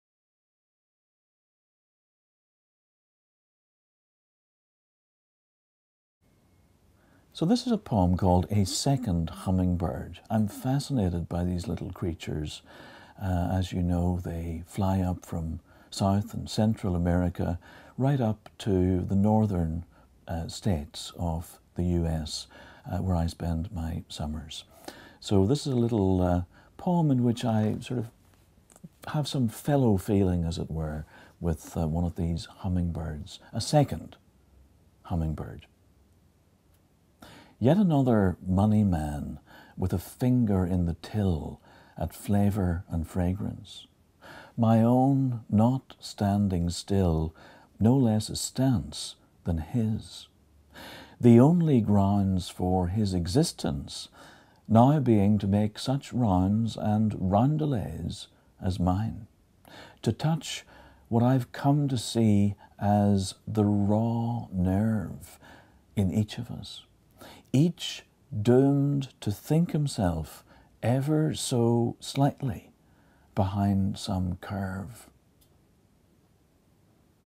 A Second Hummingbird by Paul Muldoon
Paul Muldoon reads from his latest poetry collection 'Maggot'.